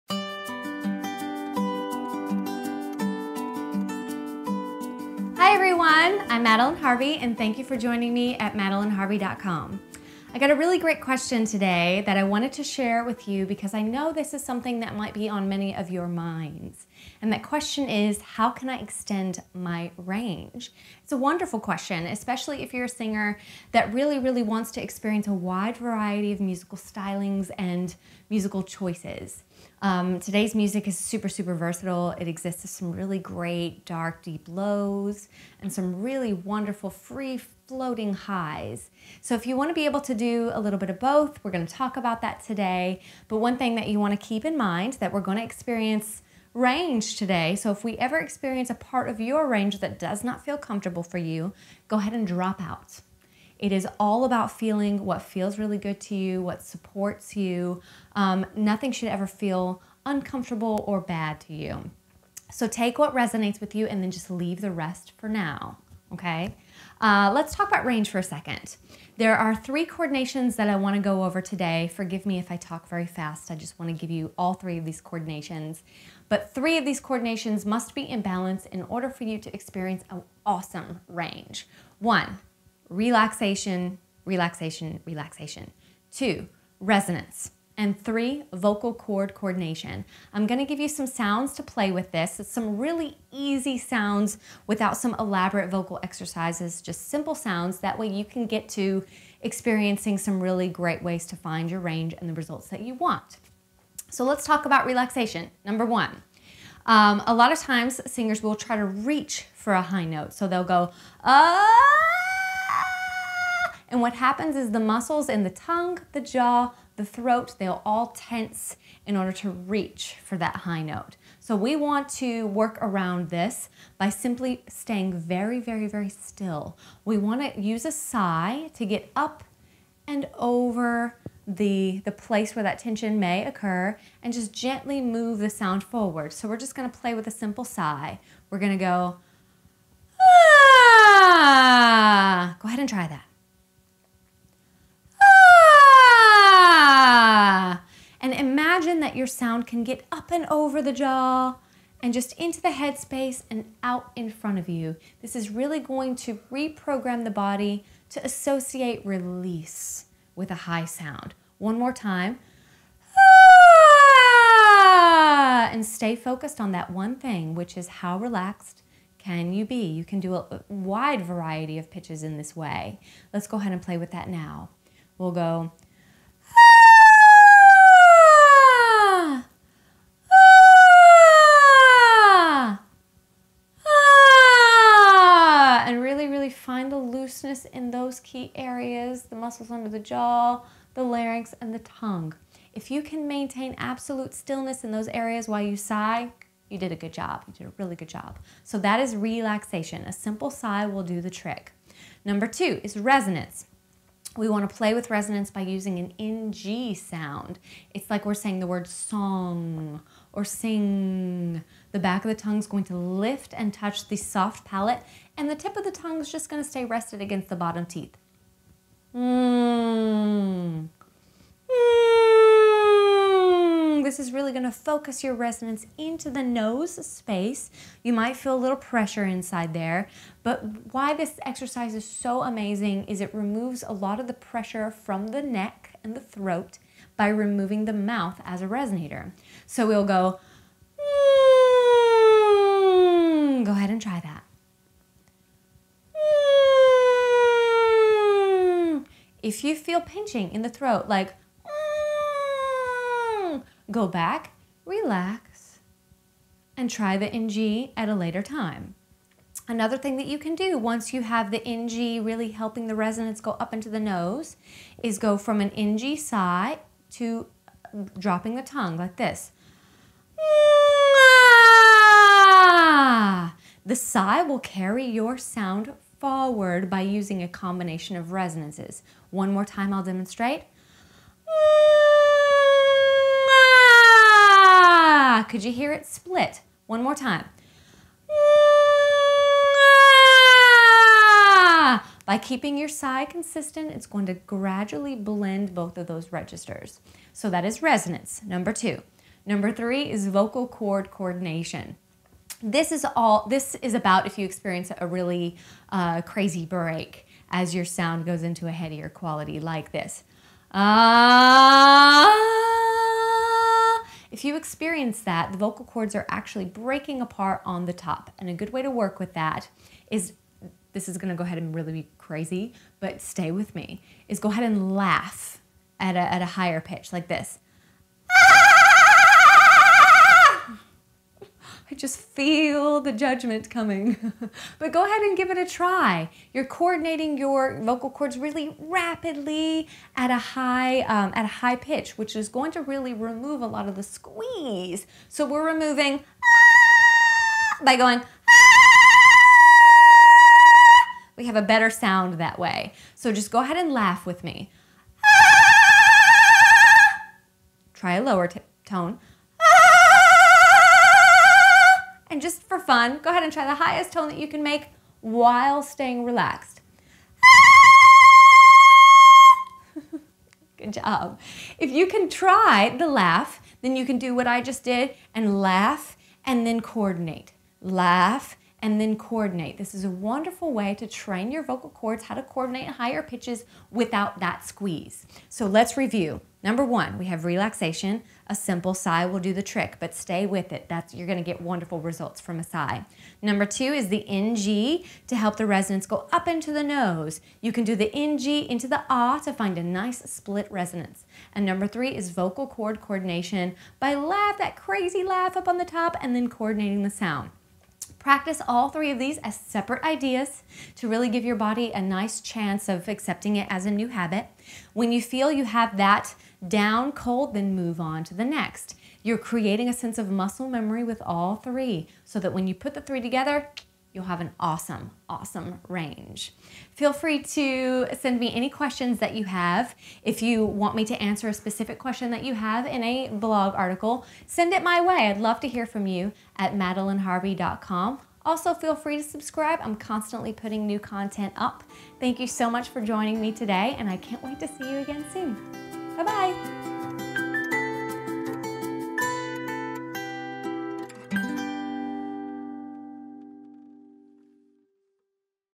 How to increase your vocal range - 3 simple exercises
I received a great question today that I wanted to share with you, because I know this is something that might be on your mind. The question is, “How can I increase my vocal range?” or “How can I extend my vocal range?”

This is a wonderful question. Especially if you are a singer that really wants to experience a wide variety of musical stylings and musical choices. Today’s music is super versatile. Artists use a mix of really great dark deep lows and some really wonderful free-floating highs. If you want to be able to do a little bit of both, then this video blog post is just for you. Today we are going to talk about how to increase your vocal range.

How to increase my vocal range
Let’s talk about vocal range for a moment. There are three coordinations that I want to go over today. These three must be in balance in order for you to increase your vocal range.

1. Relaxation
2. Resonance
3. Vocal cord coordination

In the video above, I’m going to give you some simple sounds to play with these three coordinations. These are not elaborate vocal exercises. They are just simple sounds that you can use on your own. They are proven to help increase your vocal range.

Relaxation
Let's talk about the first of the three coordinations that will help increase your vocal range, Relaxation. A lot of times of singers will try to reach for a high note. What happens is that the muscles in the tongue, the jaw and the throat will feel all kinds of tension in order to reach for that high note. We want to work around this by simply staying very very very still. We want to use a sigh to get up and over the place with that attention may occur. We just want to gently move the sound forward. You want to imagine that your sound can get up and over the jaw, into the head space, and out in front of you.

This is going to reprogram the body to associate release with a high sound. When you do the sigh exercise, stay focused on how relaxed you can be. You can do a wide variety of pitches if you can maintain absolute stillness in your jaw, your larynx, and your tongue while sighing.

Resonance
Now let’s move on to the second of the three coordinations that will help increase your vocal range. Here we want to play with Resonance by using an NG sound, like we’re saying the word “song” or “sing”. The back of your tongue is going to lift and touch the soft palate. The tip of the tongue is just going to stay resting against the bottom teeth. This is really going to focus your resonance into the nose space. You might feel a little bit of pressure inside there.

This exercise is so amazing because it removes a lot of the pressure from the neck and throat by removing the mouth as a resonator. If you feel pinching in the throat, go back and relax. Then try the NG at a later time.

After you have the NG really helping your resonance into the nose space, go from an NG sigh to dropping the tongue. The sigh will carry your sound forward by using a combination of residence spaces. By keeping your sigh consistent, you will gradually blend both of those registers.

Vocal Cord Coordination
The last of the three coordinations that will help increase your vocal range is Vocal Cord Coordination. This will help you specifically if you are experiencing a really crazy break as your sound enters your headier quality. If you experience that, vocal words are actually breaking apart on the top. A good way to work with that is to do a crazy laugh at a higher pitch. Go ahead and give it a try. You are coordinating your vocal cords really rapidly at a high pitch, which is going to remove a lot of the squeeze. Play with different pitches for a better sound. Try to laugh and then coordinate the pitch. This is a wonderful way to train your vocal cords to coordinate at higher pitches without squeezing.

Feel free to send me any questions that you have. For more info on how to increase your vocal range, check out I'm constantly putting up new content.

Thank you so much for joining me today and I can’t wait to hear you sing.